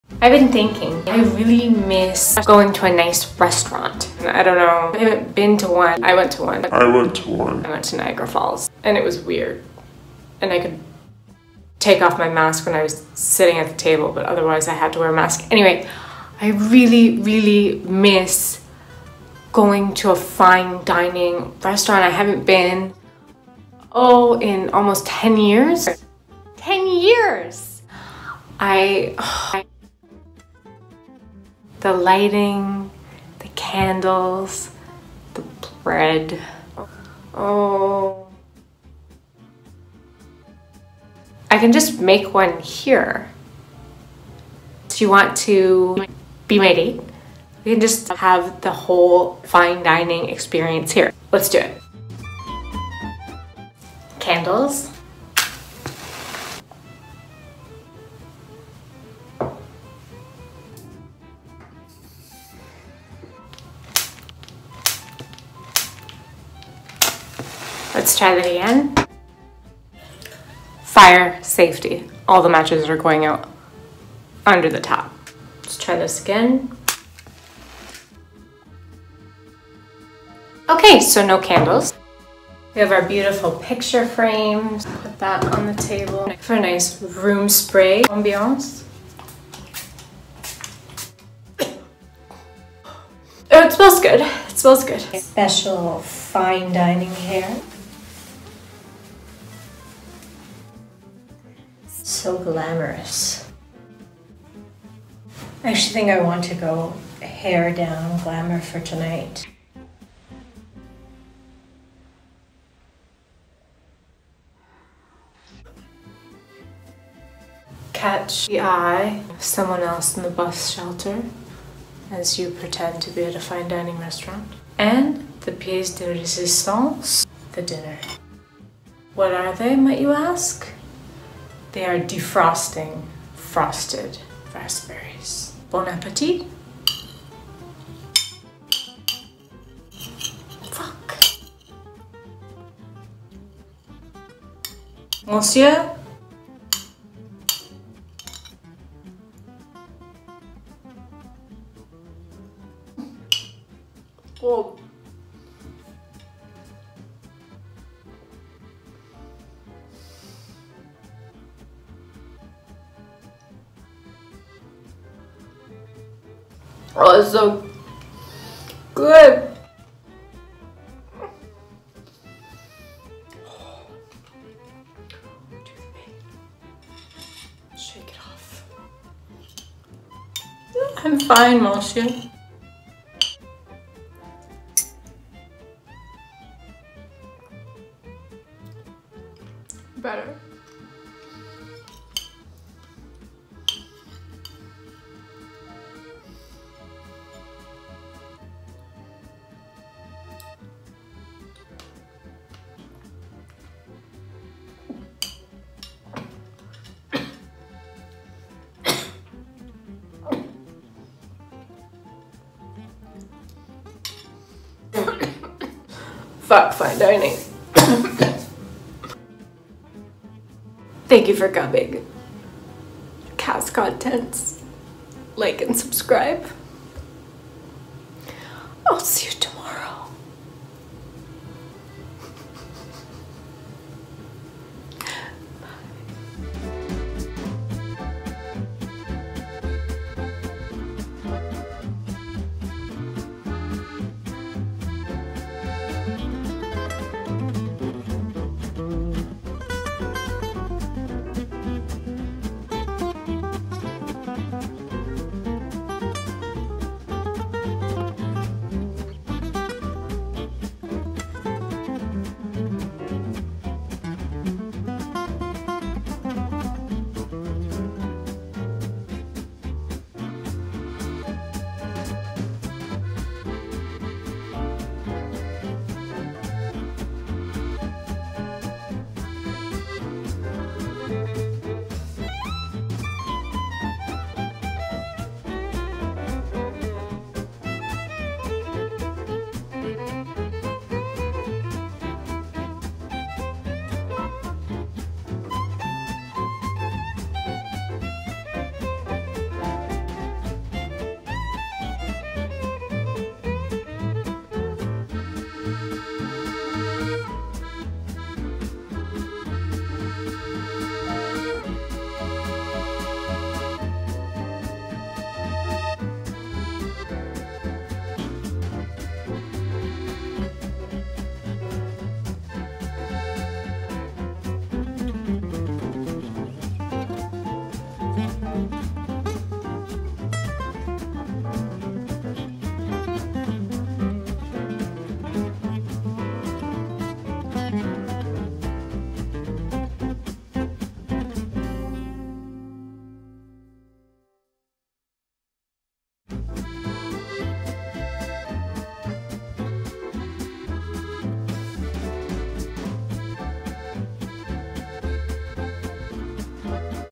How To Date (He'll Be Begging)
Will you go out to dinner with me, I mean, will you be my date? Let’s make a fine dining experience at home! Let’s go!!

I would love it if you might consider adding my on

As all a this stuff helps me grow and with that helps me get out more videos for all of you, which is what I LOVE TO DO!!!